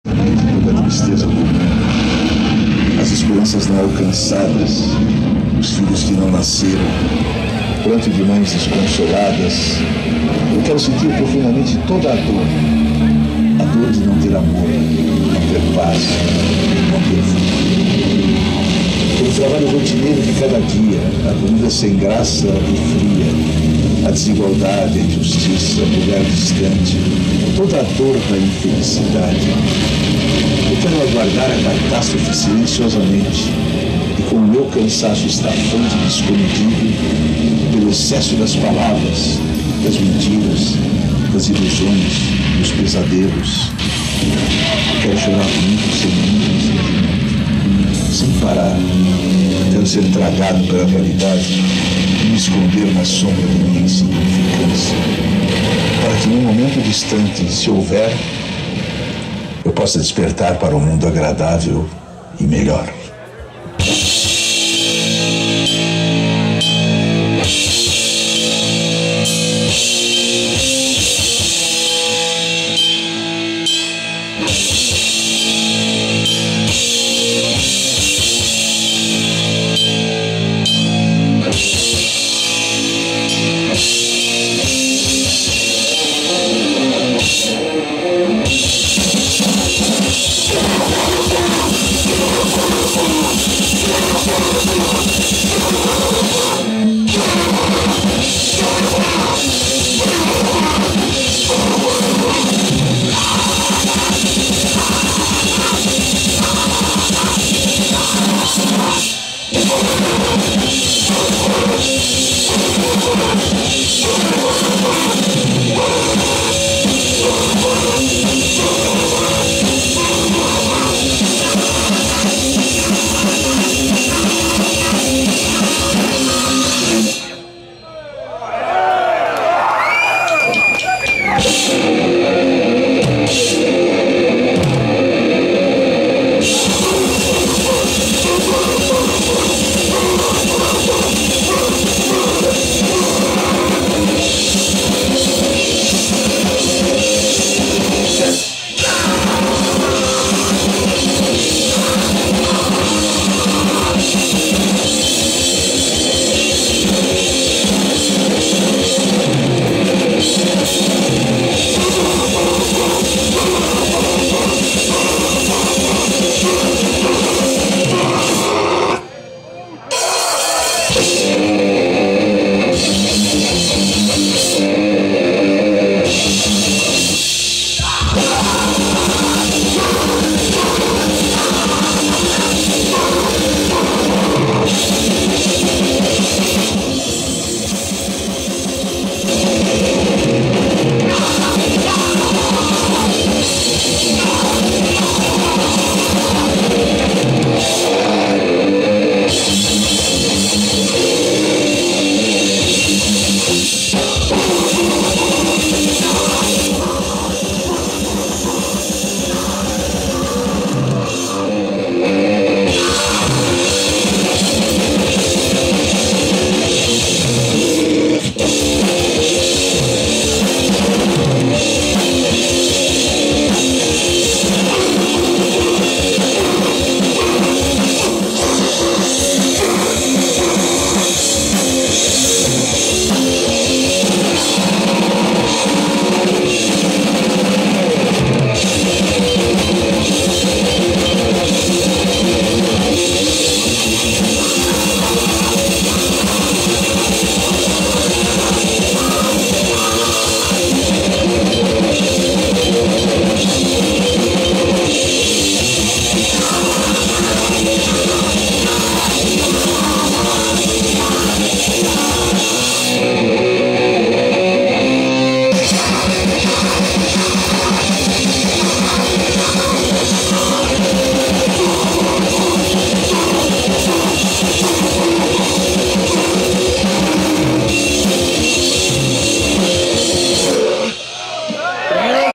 SUB CUT - SÃO CAOS FESTIVAL 2, BRASIL - 10.10.15
Wallas Tösko Video - Sub Cut  tocando no São Caos Festival 2 em São Carlos no dia 10/10/15 // Sub Cut playing at São Carlos Festival, in São Carlos city, Brasil